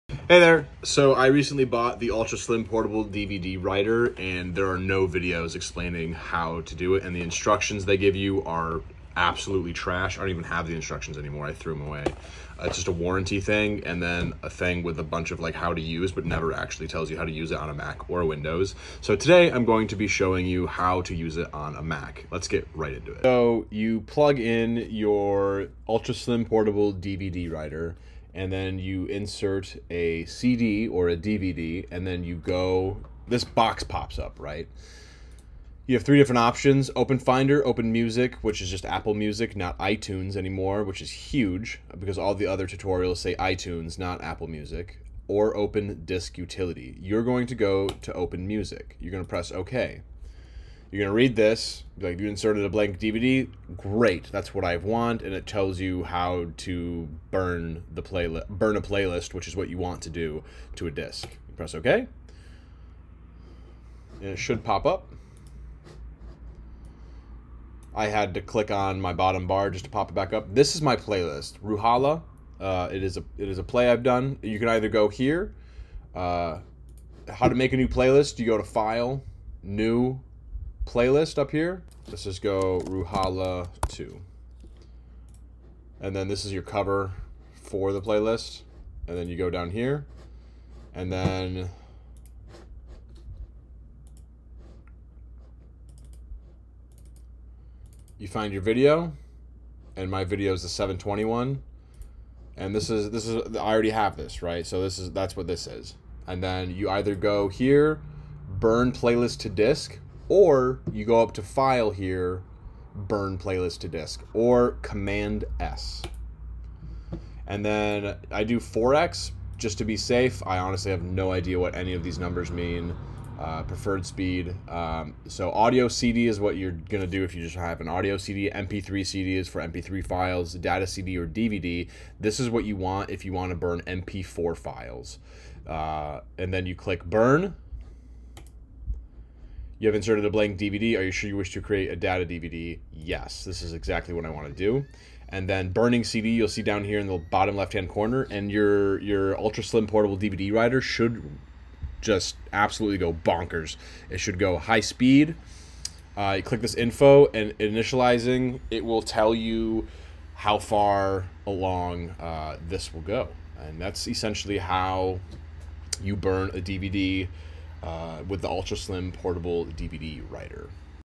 How to Burn a DVD with LG ultra slim portable DVD writer - MacBook Pro
LG Ultra slim portable DVD Writer doesnt come with instructions for MAC or PC so I hope this helps you out. This tutorial is for MAC because I don't have a PC anymore - my apologies.

Make sure the disk is blank and has nothing on it.

Add as much as you want to the playlist in Apple music before you add the entire playlist to the DVD.

Cheers!